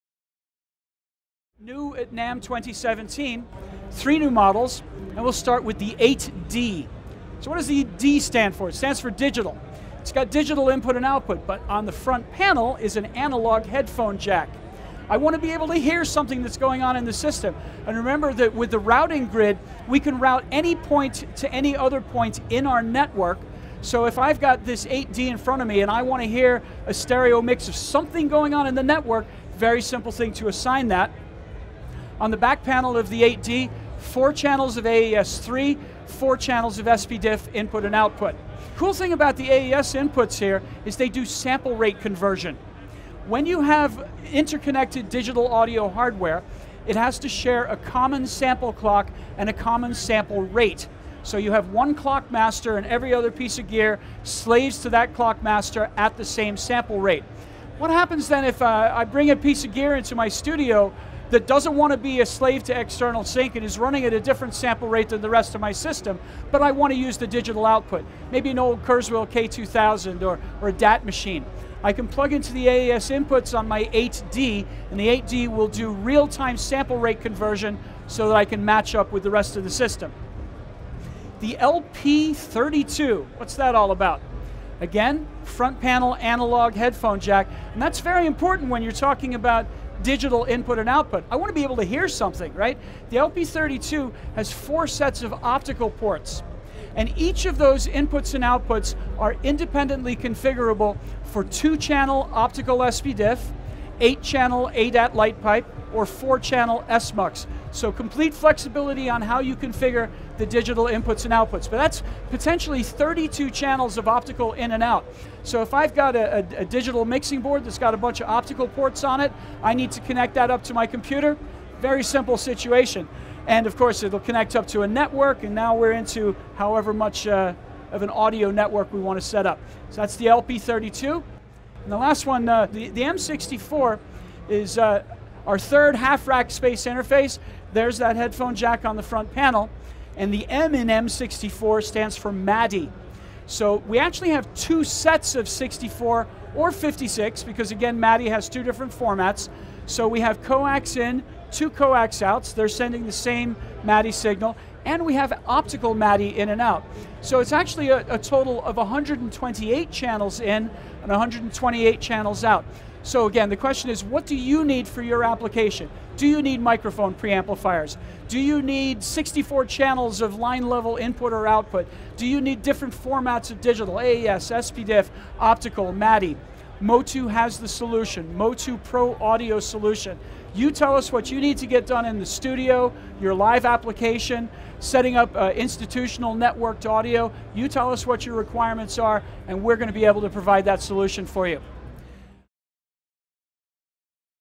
NAMM 2017: three new digital audio interfaces
MOTU's "Magic Dave" Roberts provides a quick overview of three new digital half-rack interfaces: